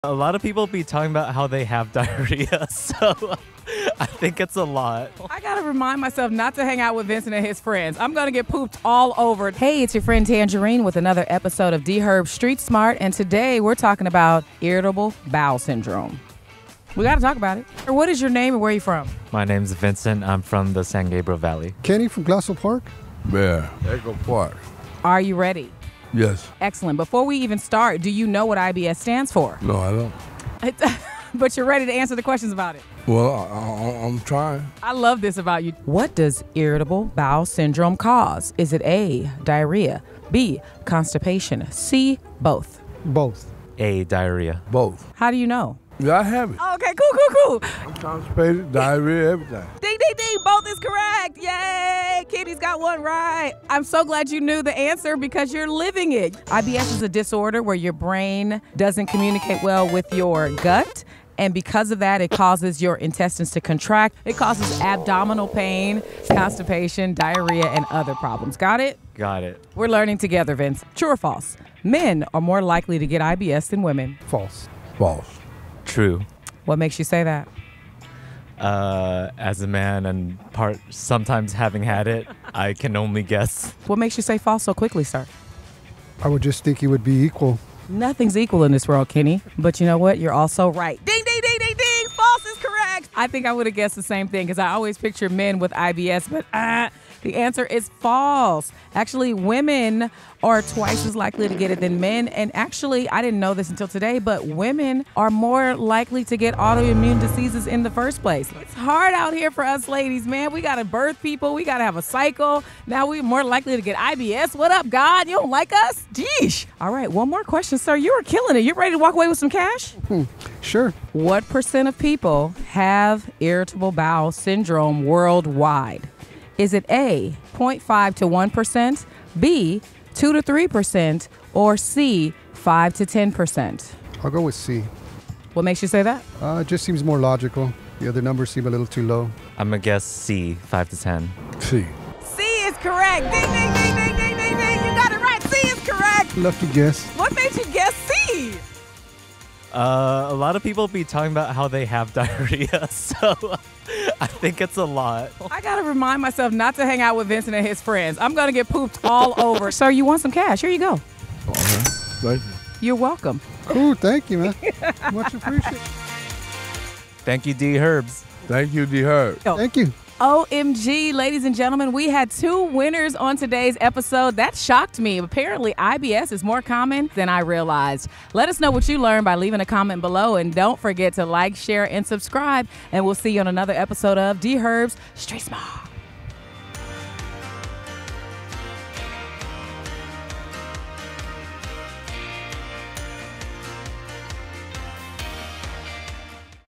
IBS and How YOU May Be Suffering From It
Irritable Bowel Syndrome, also known as IBS, is a lot more common than you think! It affects 10-15% of the global population.

⏰ Chapters ⏰
0:00 Intro
0:35 What does Irritable Bowel Syndrome cause?
1:10 Men are more likely to get IBS than women.
2:01 What percent of people have IBS worldwide?
2:45 Outro